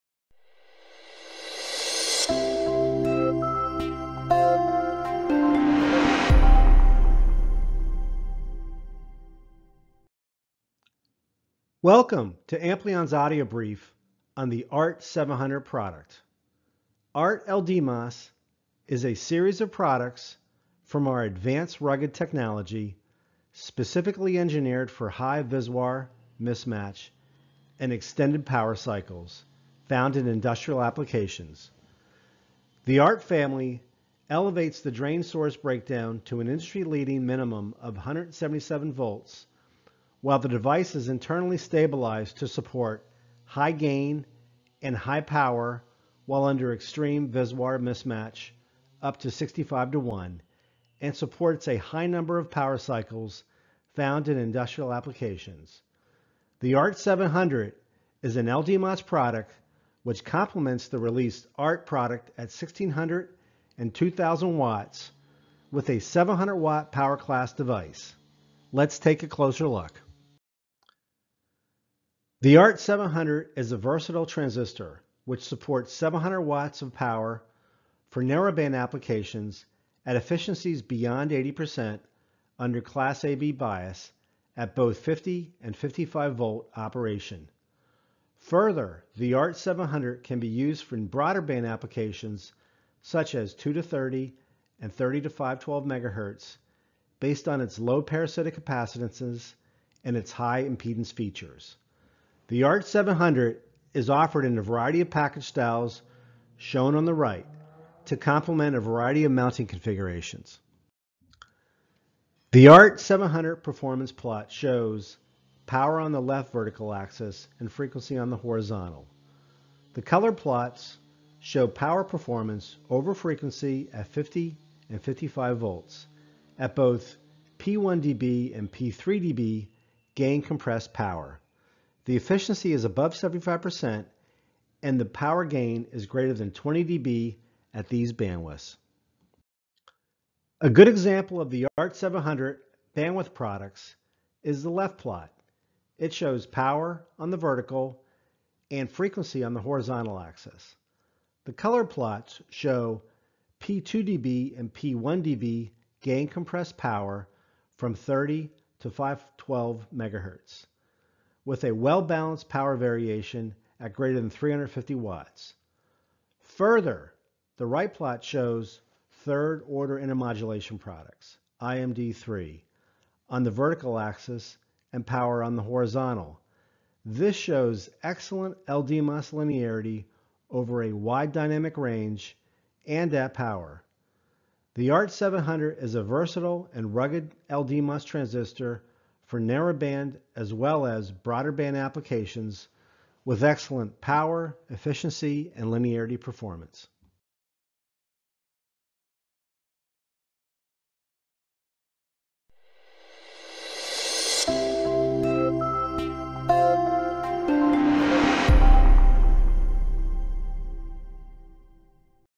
The “Application Versatility” of the ART700FH(S) 700 W, 50 V LDMOS
ART LDMOS is a series of products from our (A)dvanced (R)ugged (T)echnology, specifically engineered for high VSWR mismatch and extended power cycles in industrial applications. The ART family elevates the drain-source breakdown to an industry-leading minimum of 177 V, while the devices internally stabilized to support high gain and high power under extreme VSWR mismatch up to 65:1, and supports a high number of high number of power cycles in industrial applications. The ART700 is an LDMOS product which compliments the release of ART products at 1600 and 2000 W with a 700 W power-class device.

Get in touch with our Sales team or our Distribution partners if you need support or have any further questions.